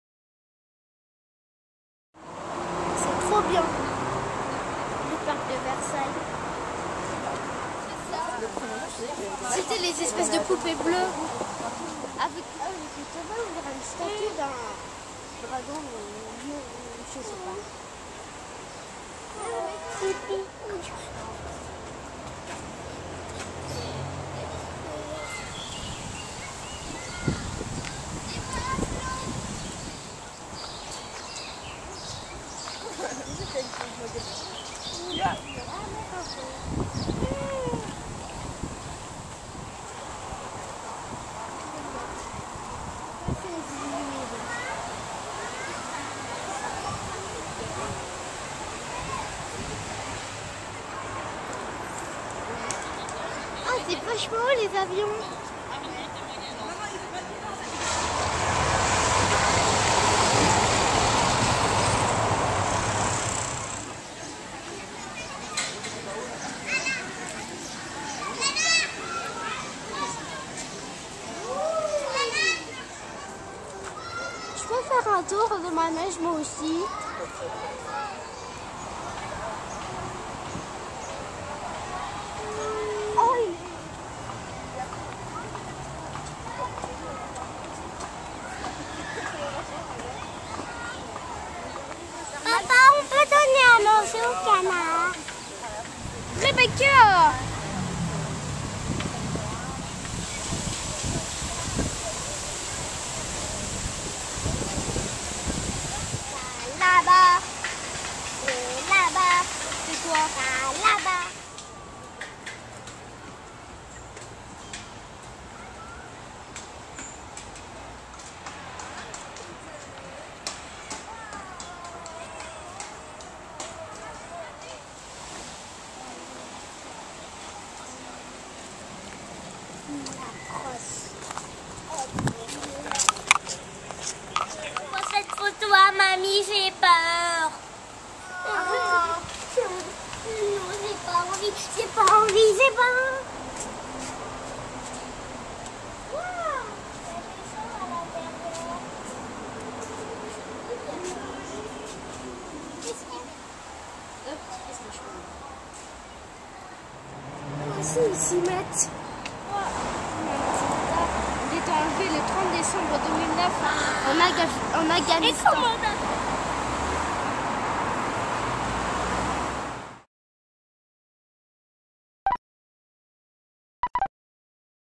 Paris, au parc Monceau, juin 2011
Le parc Monceau est peu visité des touristes, il est pourtant entouré de très belles demeures